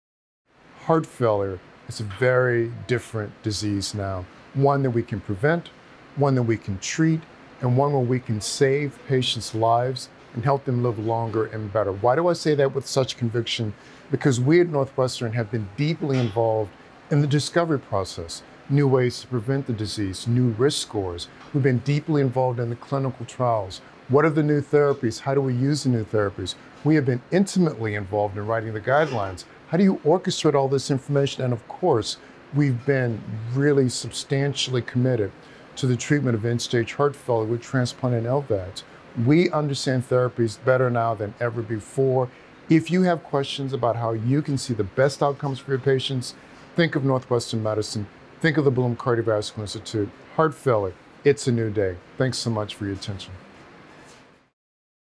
Heart Failure: Discovery, Prevention, Genetics and Clinical Trials
Clyde W. Yancy, MD, chief of cardiology at Northwestern, discusses the continued commitment of Northwestern to the treatment of heart failure.